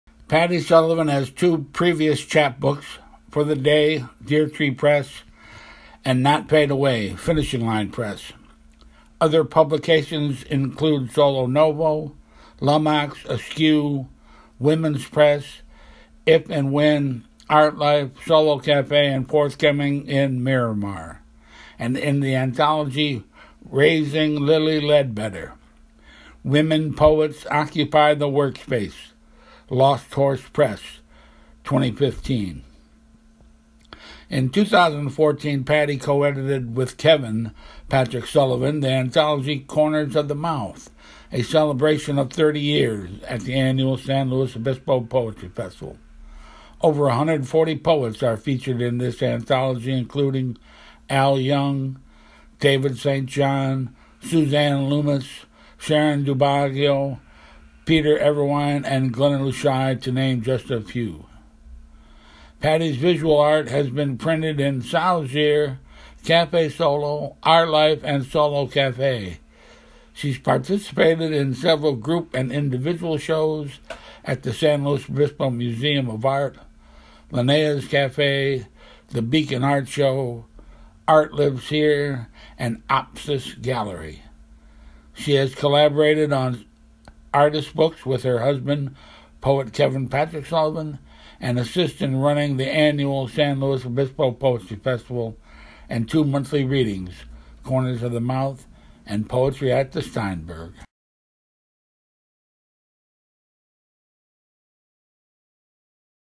Patti's Bio
Published in: At the Booth Memorial Home For Unwed Mothers 1966 by Patti Sullivan (Evening Street Press, 2015)